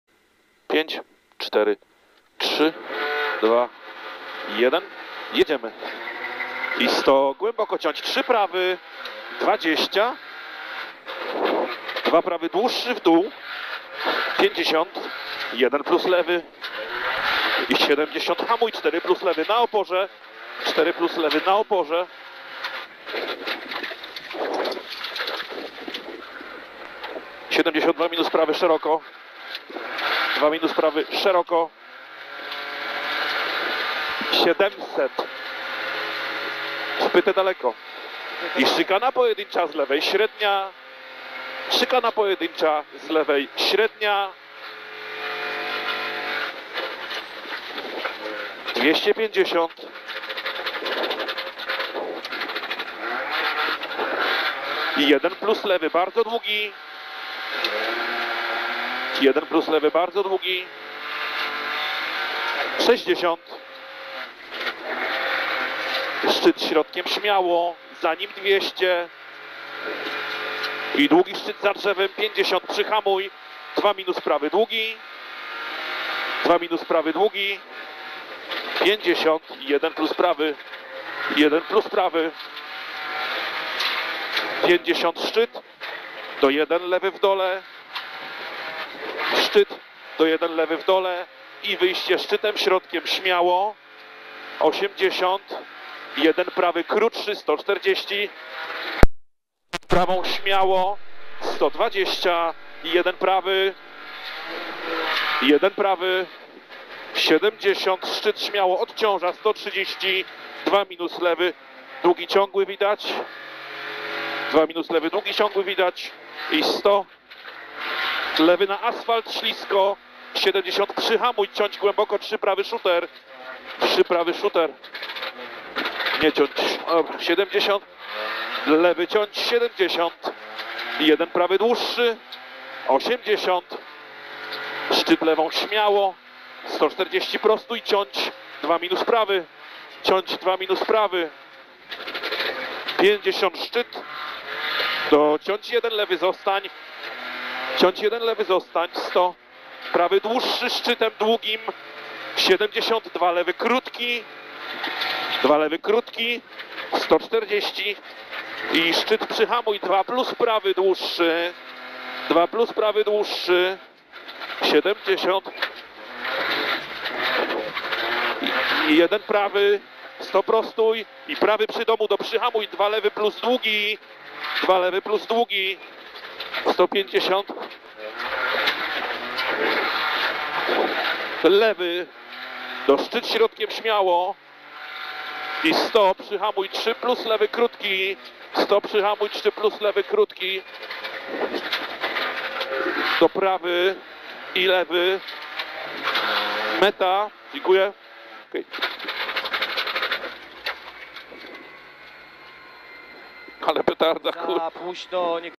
Zemantija Rally 2021 - SS 3 AutoPartner on-board
Odcinki rajdu Żmudzi (Zemantija) to absolutna petarda. Są szybkie, droga się  nie niszczy i dają dużo radości z rajdowania. Wrzucam on-board pierwszego odcinka rajdu, który pomimo paru błędów wygraliśmy w klasie 4. To była duża przyjemność sportowa brać udział w tym wydarzeniu rajdowym.